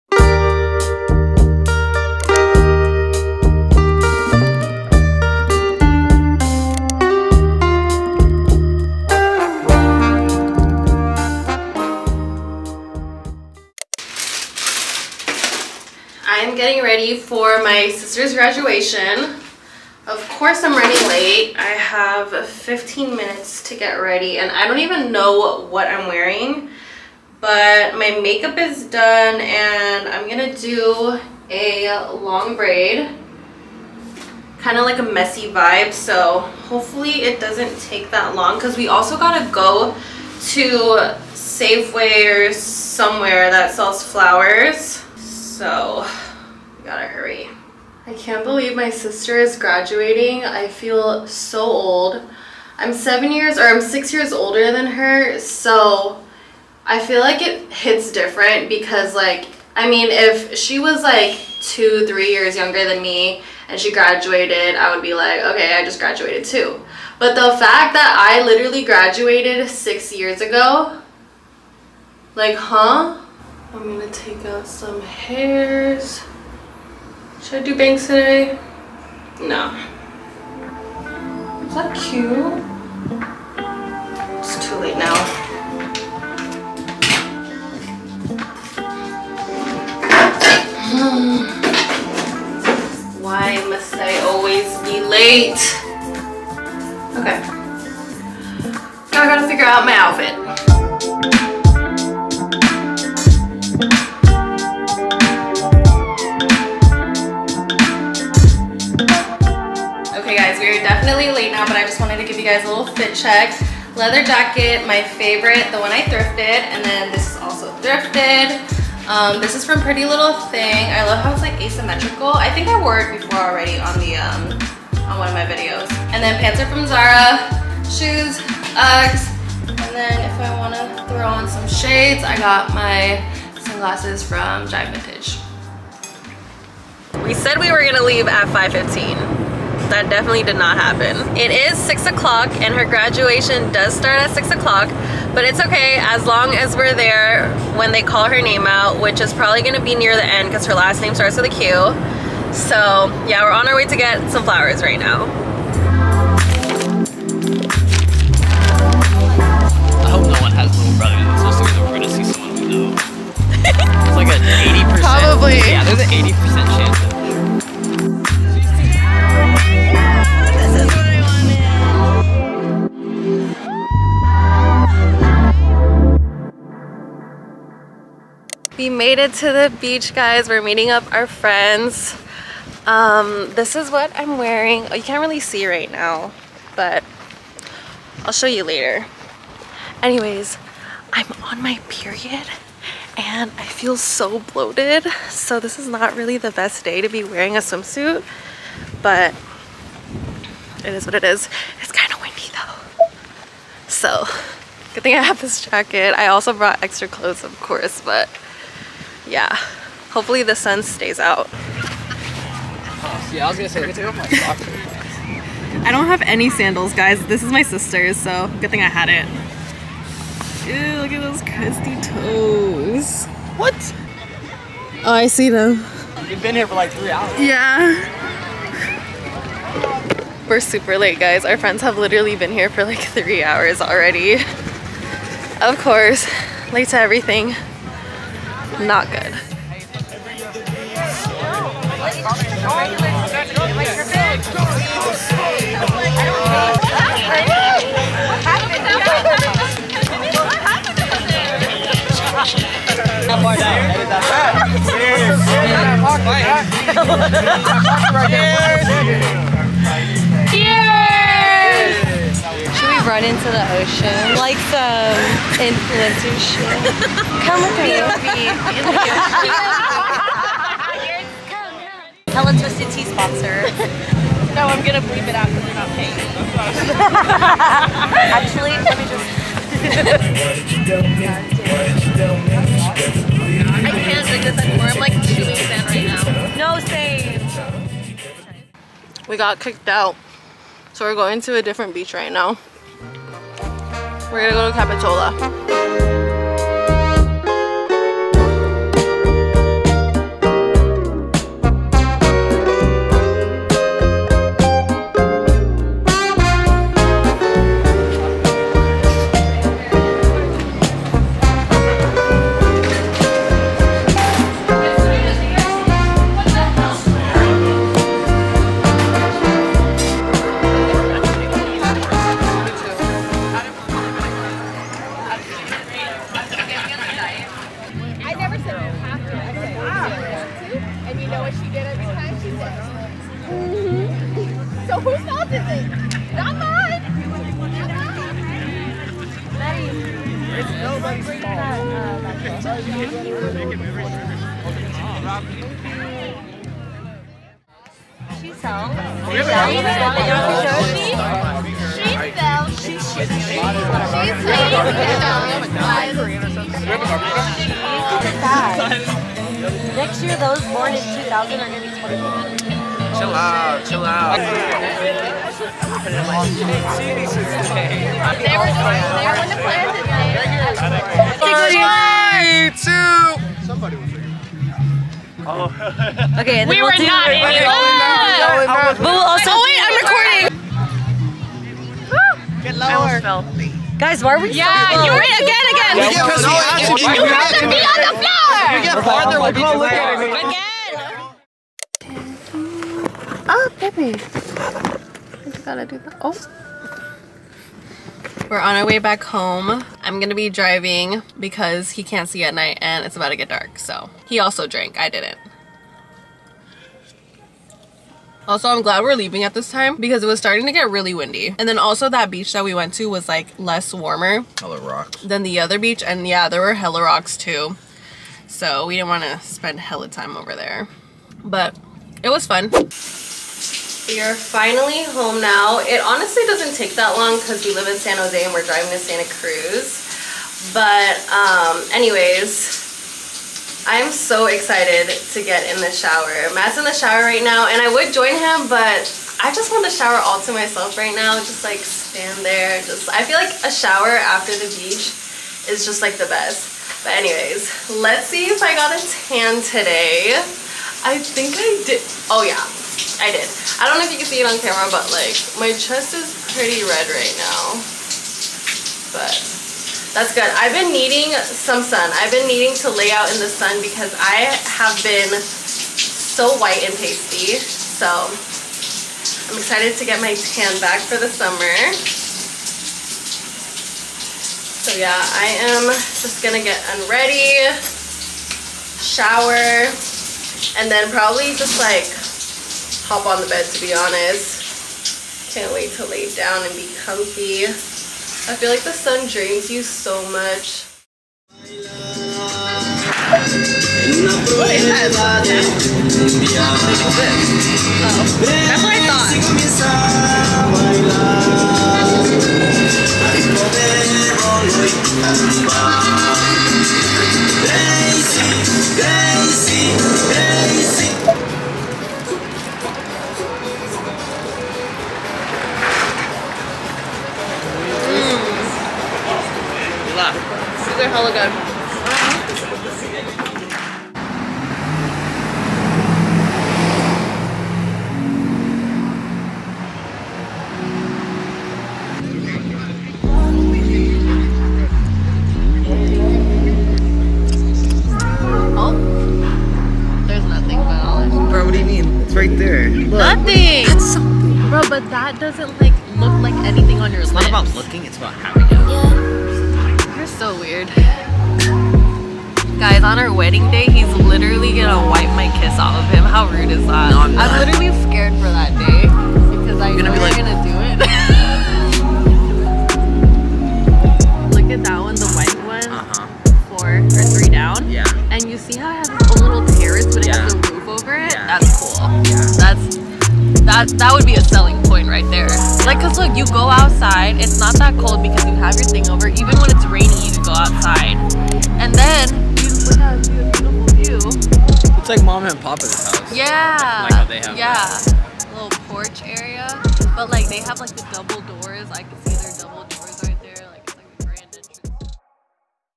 weekend in my life vlog | beach days, graduations, celebrations
This weekend the sun came out so my friends and i all went to the beach! We also go to my sister's graduation and matt and i celebrate our anniversary ‎♡‧₊˚

00:00 Introduction
00:16 Sister's Graduation
02:58 Beach Day
08:12 Anniversary in SF